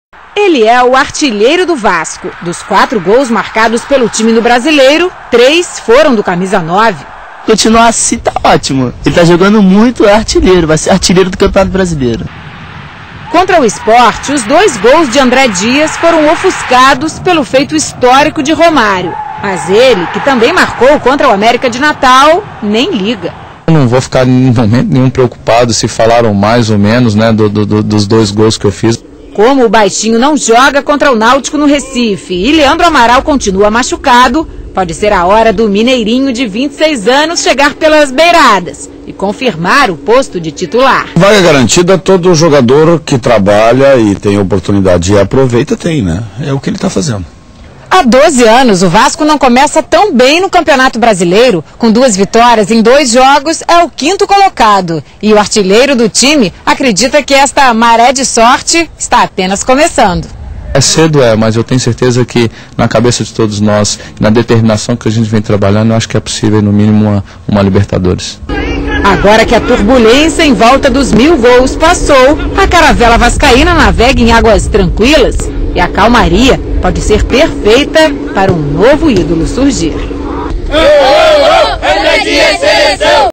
23/05/2007 - Entrevista com André Dias
Entrevista com André Dias, artilheiro do Vasco no Brasileiro